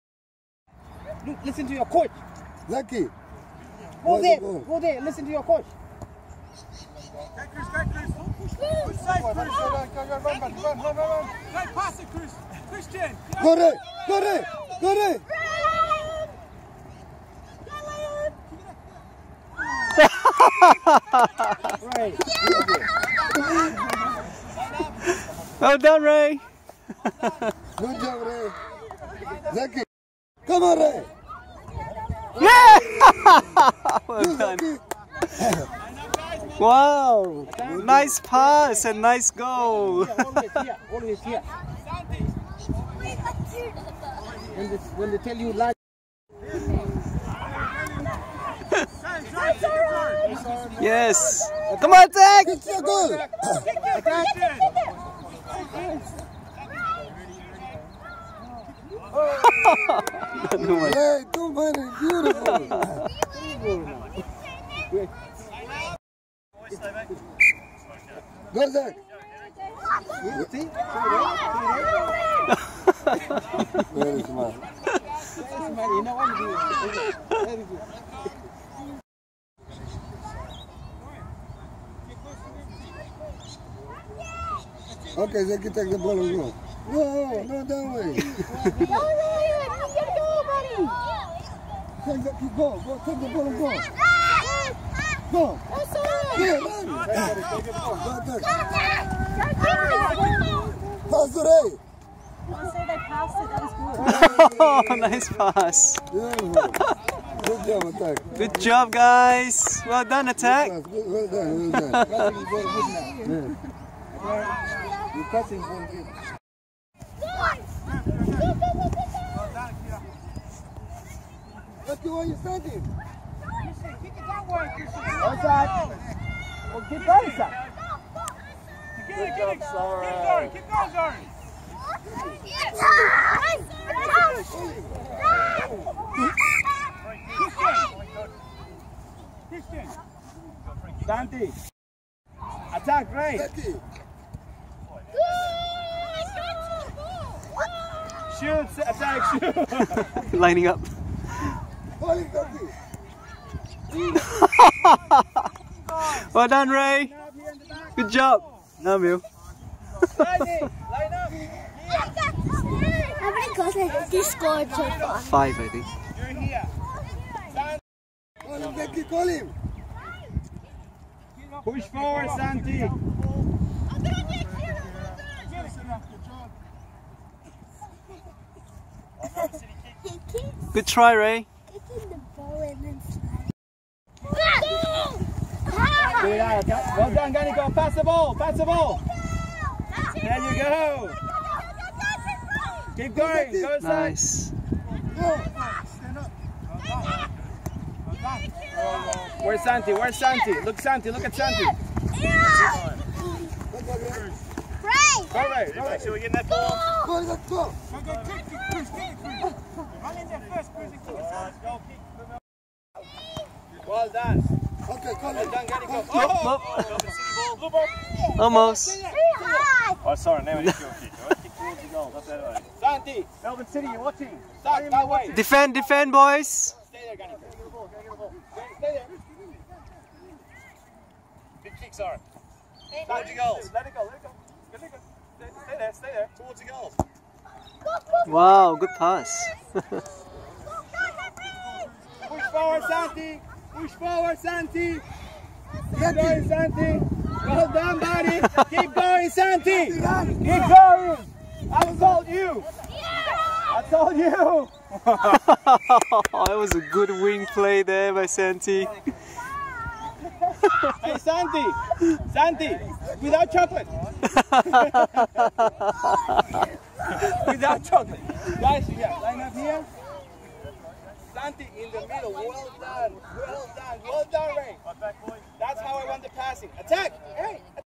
Rei Goh - 6 goals - Point Cook Jets Lightnings vs Melbourne City (6:0) - 10/06/2018
Rei's personal best performance today as he captained the Point Cook Jets Lightnings to a 6-0 victory over the Melbourne City Juniors in the Under-7s Miniroos League.

Well done Rei and the Lightnings, it was a great performance!